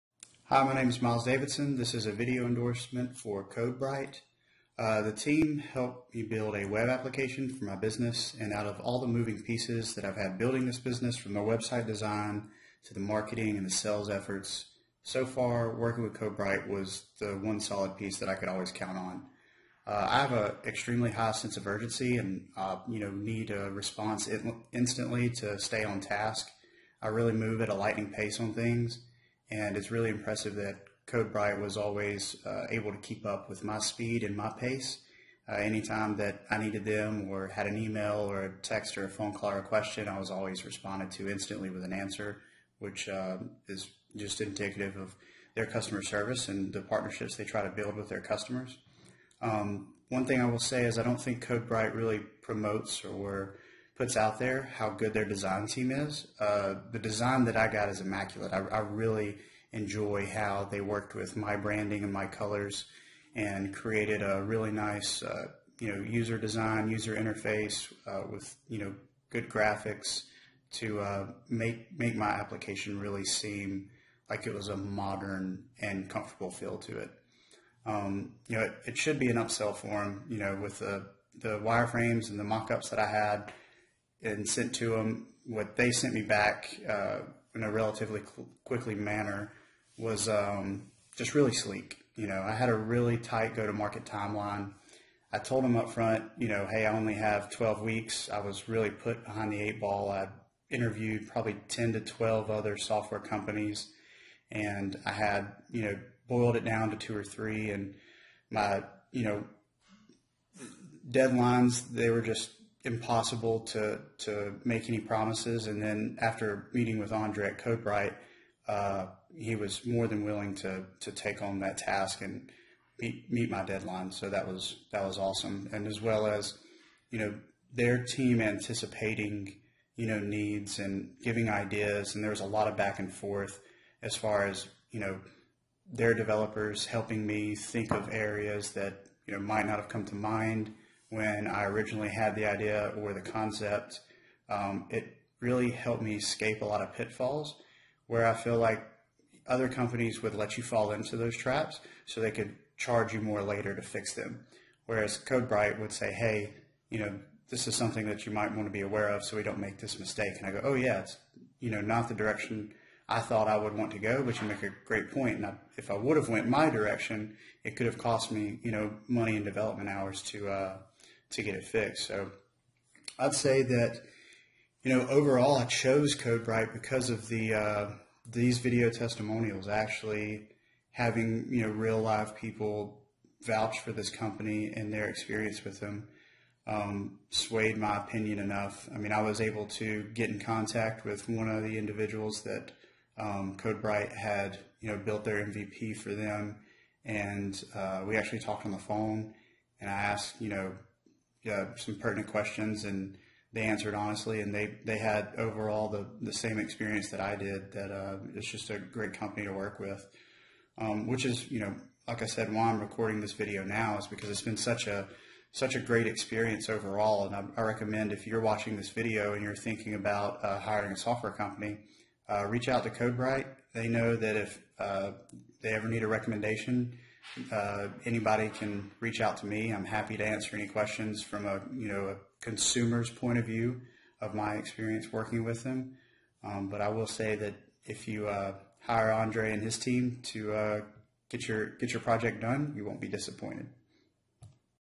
Video Review From Miles Davidson For CodeBright
In frame Miles Davidson describing his experience working with CodeBright- " Andre and his team has been great from the start of the journey, The success credit goes to him and his team they have been together through thick and thin always trying to put in their best efforts. Can't be enough thankful. "